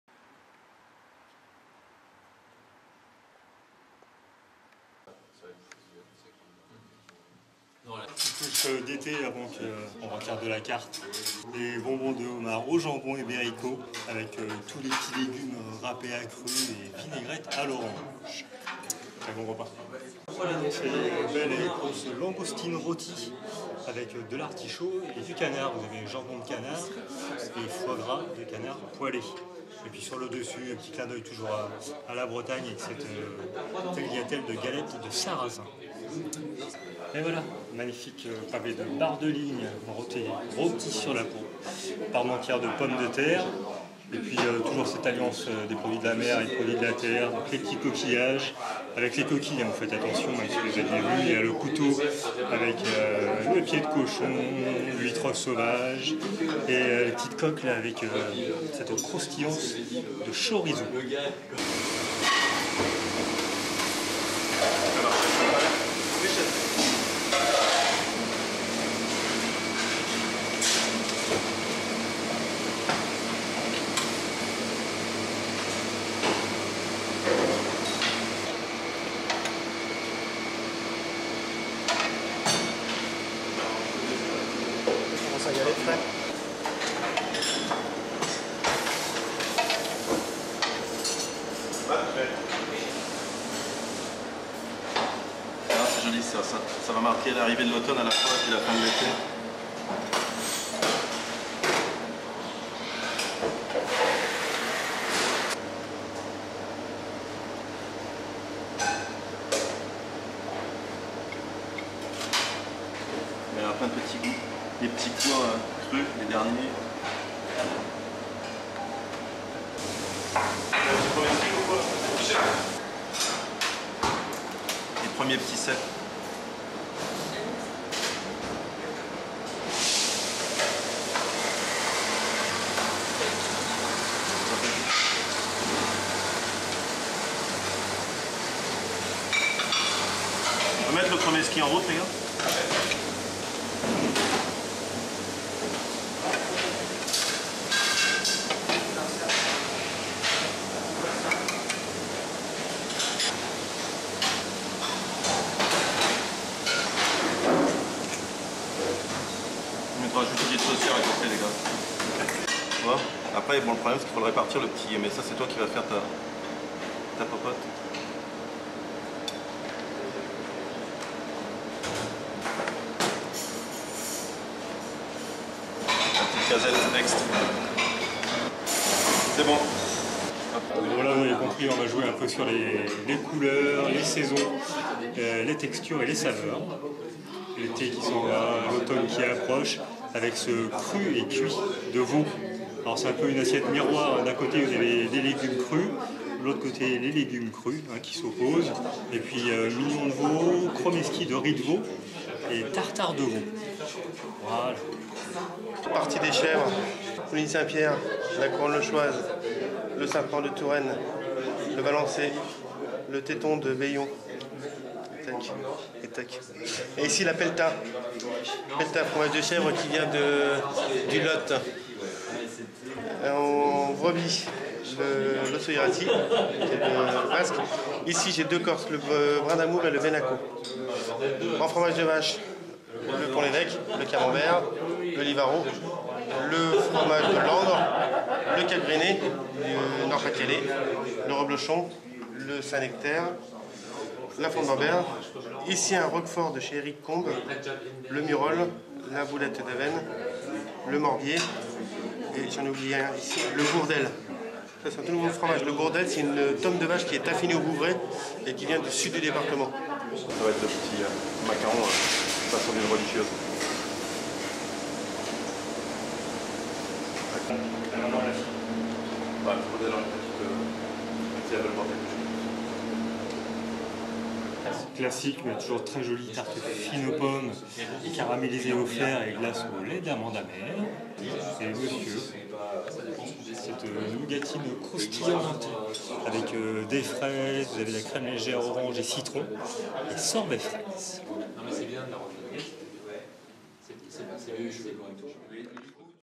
Déjeuner aux Hautes Roches * - 13 septembre 2013
Cette table est devenue au fil de ces dernières années, l'une de mes favorites, et je ne peux plus concevoir d'année culinaire sans au moins y faire une visite annuelle. Service impeccable, proche sans être familier, détendu et efficace, La cuisine de Didier Edon, secondé avec maitrise, grand talent et grâce par la radieuse Barbara qui en plus maintenant s'occupe des desserts, associe avec bonheur les produits de la terre et de la mer, une particularité bien bretonne que j'apprécie beaucoup. Ah ces bonbons de homard au jambon ibérico, ces langoustines, artichauts et tagliatelles au blé noir, ce bar agrémenté de coquillages, de pied de cochon et de chorizo, et cette déclinaison sur le veau, en cromesquis, en tartare et en mignon, le tout  escorté de légumes crus et cuits (n'est-ce pas Fred !) du meilleur effet. Et que dire des accords vineux concoctés par le sommelier ! On en redemande ...